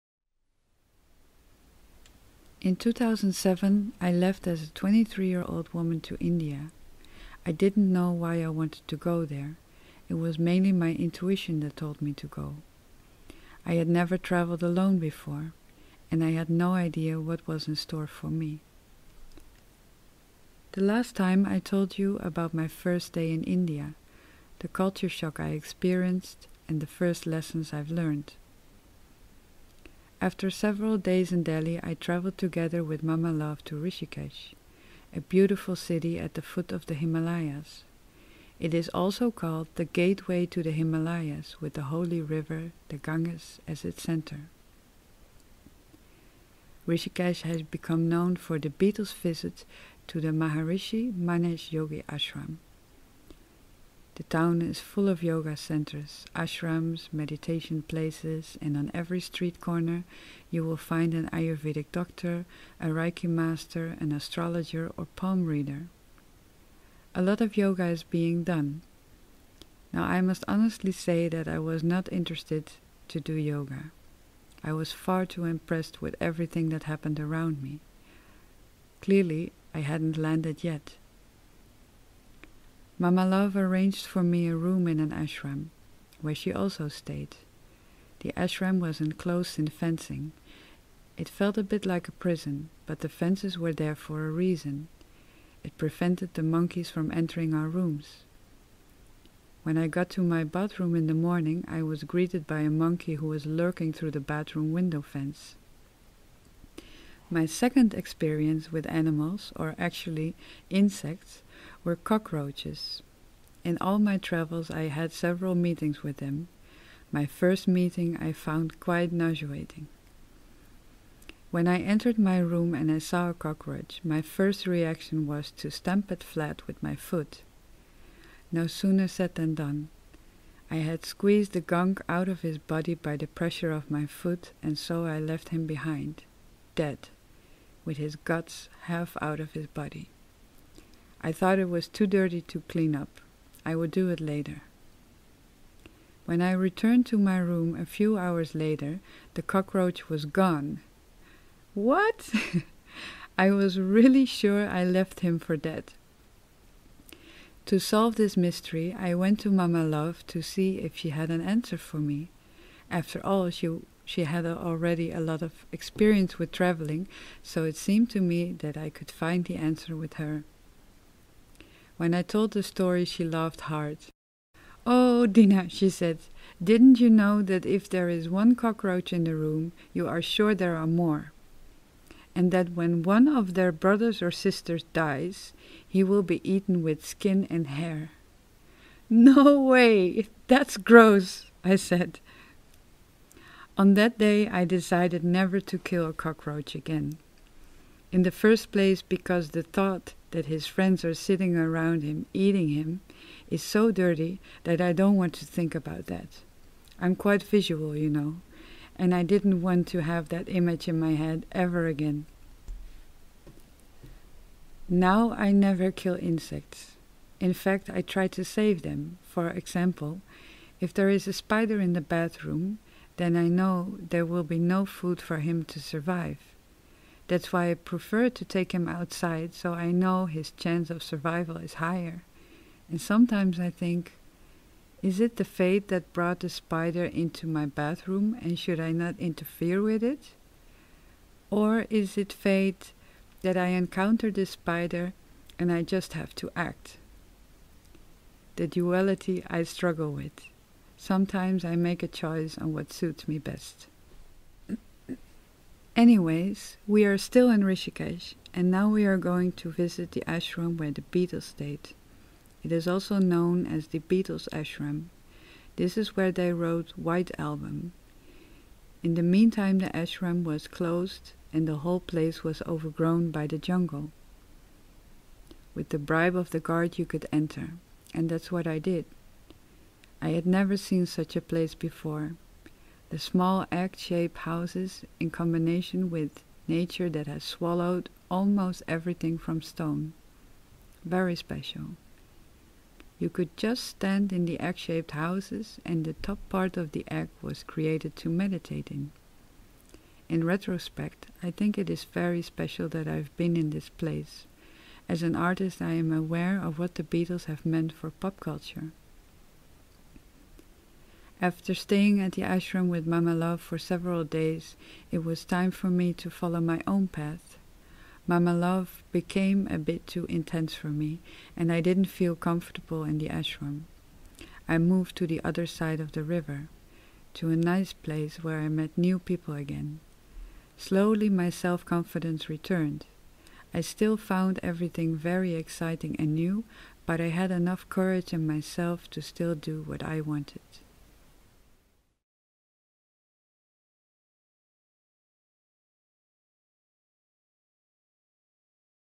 PODCAST - Rishikesh
In 2007 I left as a 23 year old woman to India. I didn't know why I wanted to go there. I had seen some pictures on the TV, but it was mainly my intuition that told me to go. I had never traveled alone before. And I had no idea what was in store for me ...